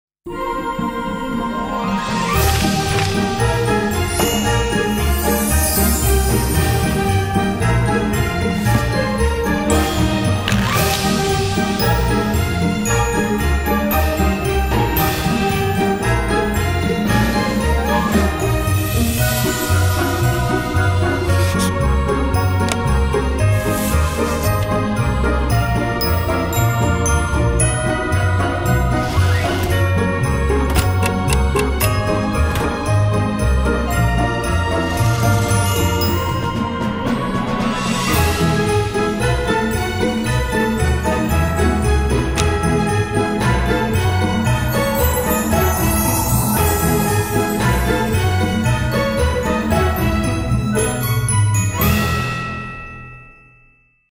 The Fastest Way to Send Letters to Santa
This one-of-a-kind activity and storybook brings together the beloved Christmas traditions of writing letters to Santa, trimming the tree, holiday baking and The Elf on the Shelf. Watch magic unfold as Santa’s Special Paper transforms your children’s full-sized letters to Santa into elf-sized messages that can be delivered straight to the North Pole by their Scout Elf!